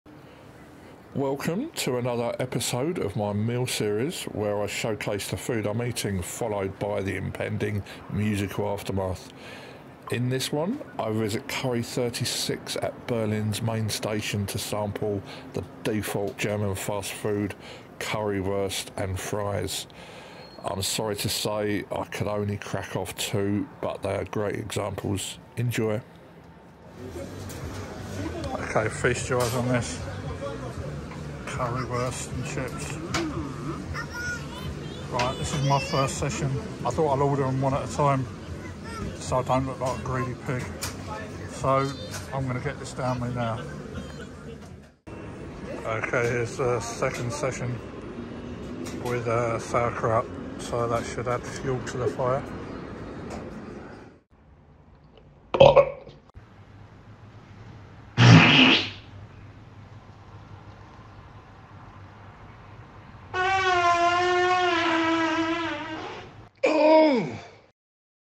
Powerful Fart Aftermath from Curry36 Berlin HBF
Here I sample the delicicous The German Goto fast food item, the Currywurst, I was at Berlin HBF anyway so I just had to try this.
My hotel room was a 5 star with a very high cieling, making the farts echo rather well.